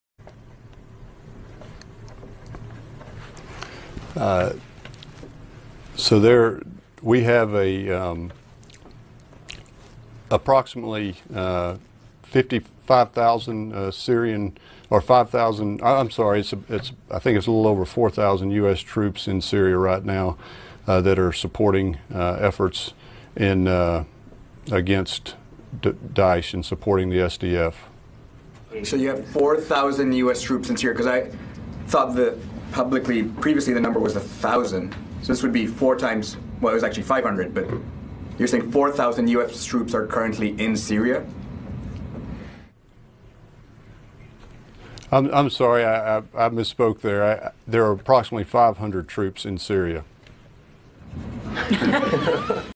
How many US troops do we have in Syria?
Army Maj. Gen. James B. Jarrard Commander of Special Operations Joint Task Force Operation Inherent Resolve fumbles with numbers when asked how many US troops we have supporting the so-called SDF (Syrian Democratic Forces) in Syria.